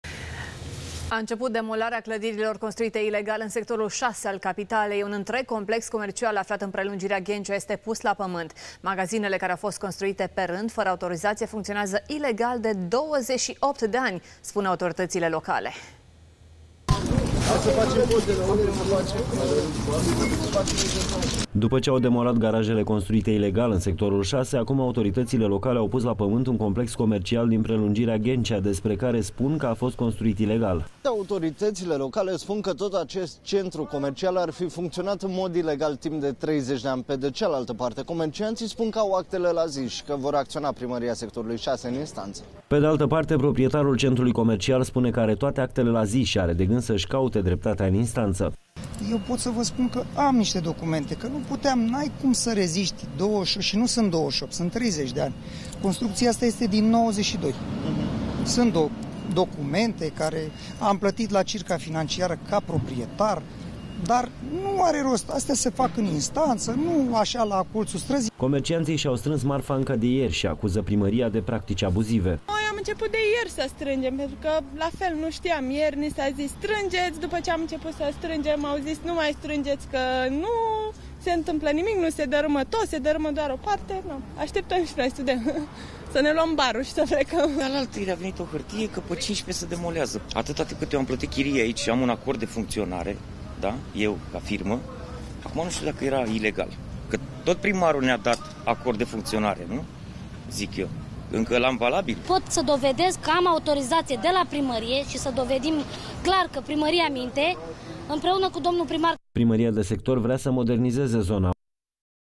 Complex comercial construit ilegal în Prelungirea Ghencea, pus la pământ de autorităţi
Un complex comercial construit ilegal în Prelungirea Ghencea a fost pus la pământ de autorităţi.